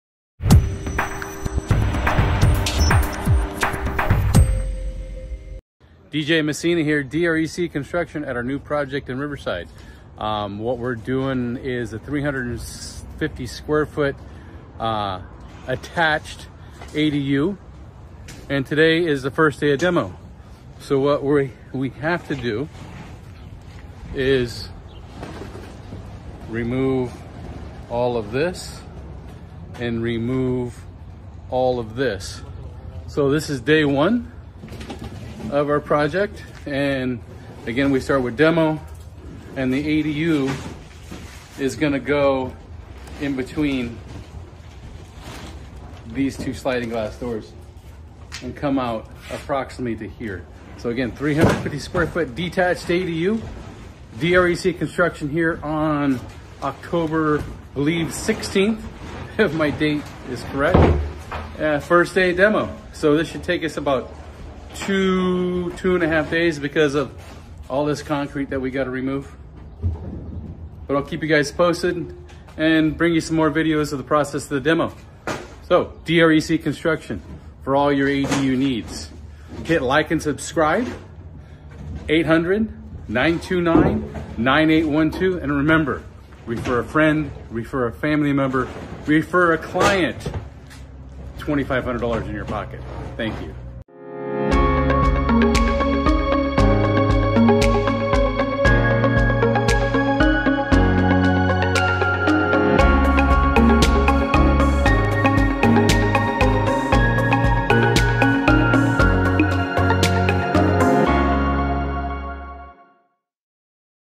ADU Riverside II | Part 1: Demo - 350 sqft Attached ADU Construction in Riverside
In this episode, witness the demolition phase as the attached ADU project in Riverside begins to take shape.
Get ready to witness the beginning of an exciting journey in "ADU Riverside II | Part 1: Demo." We're embarking on the construction of a fantastic 350 sqft attached ADU in Riverside, and the first step? Demolition! Join us as we tear down old walls and clear the way for something new and exciting. It's the start of a transformation, and we're thrilled to have you along for the ride. 🏠🚧

DREC Construction Services, Inc.